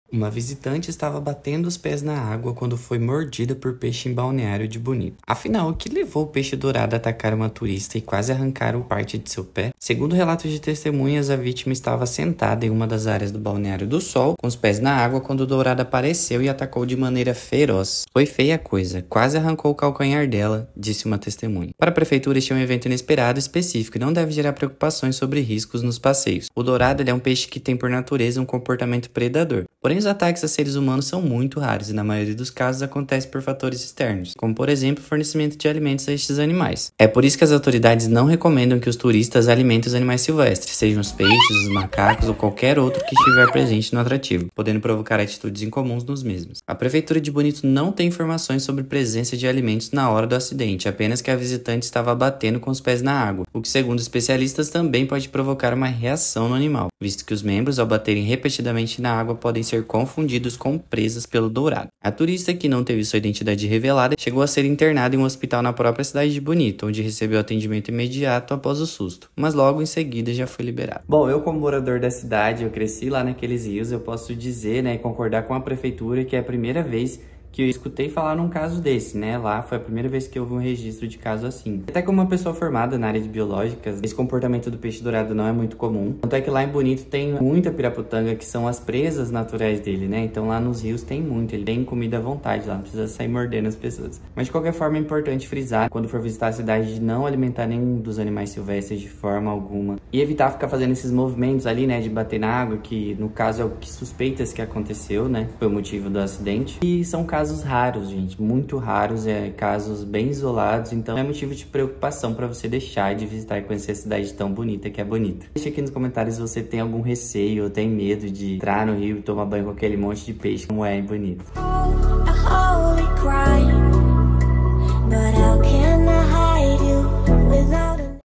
ATAQUE INESPERADO: Turista é MORDIDA POR PEIXE dourado em balneário de Bonito no Mato Grosso do Sul
Durante uma visita ao Balneário do Sol, em Bonito, localizado a 297 quilômetros de Campo Grande, uma mulher foi surpreendida ao ter seu pé mordido por um peixe dourado na quarta-feira (10). O balneário divulgou uma declaração informando que o guarda-vidas presente no local prestou assistência à turista e solicitou ajuda dos bombeiros, que providenciaram o transporte da vítima para o hospital de Bonito. "Felizmente, ela está recebendo o tratamento necessário e está em processo de recuperação", declarou parte do comunicado.
Fonte: MidiaMax notícias.
Imagens: acervo pessoal; Prefeitura de Bonito e MidiaMax.